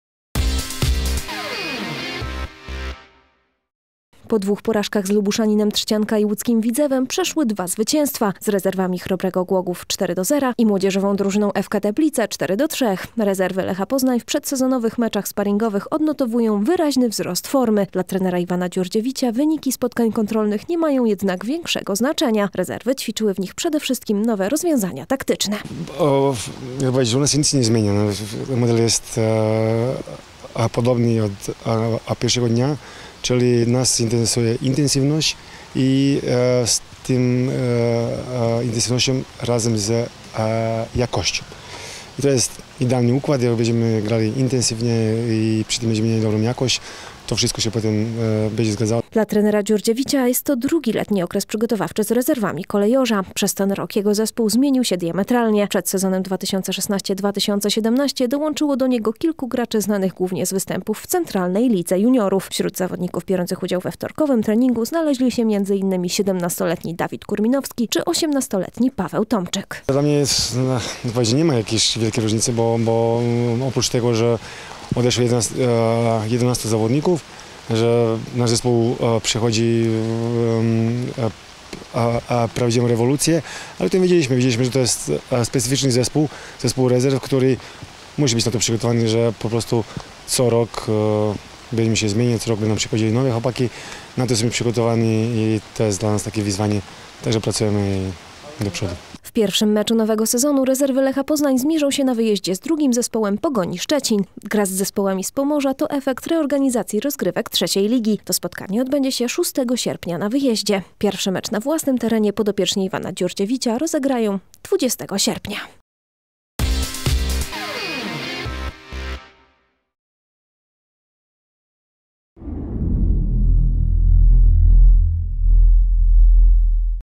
Intensywność i jakość
Trener Ivan Djurdjević ma pomysł na odmieniony i odmłodzony zespół rezerw. Nowy sezon zreformowanej III ligi startuje w najbliższy weekend. Kolejorz zagra na wyjeździe z Pogonią II Szczecin.